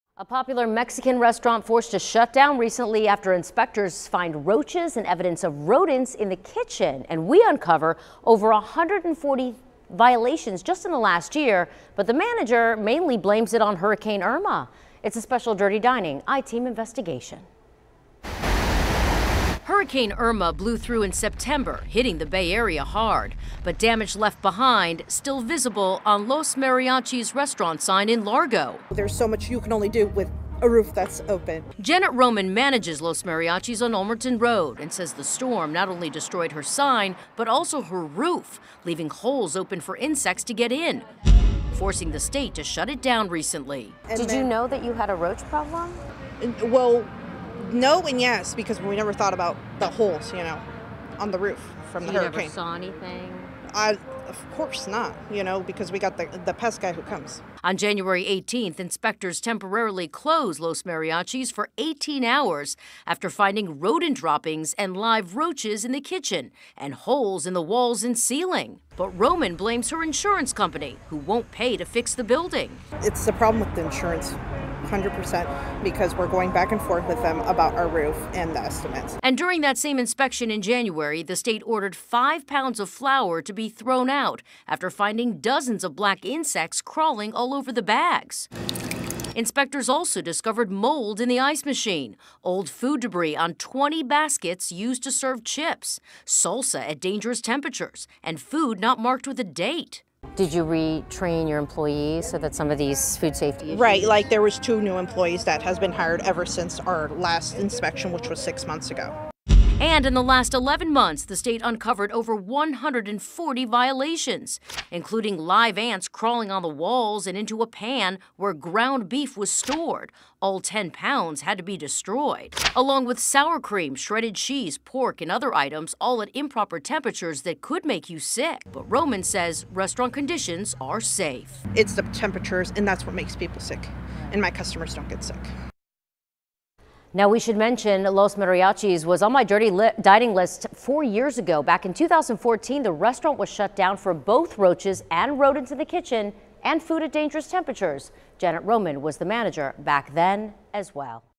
Dirty Dining: Los Mariachi's forced to shut down for roaches, rodents in the kitchen
A popular Mexican restaurant had to close its doors for 18 hours after inspectors found live roaches and evidence of rodents inside the kitchen.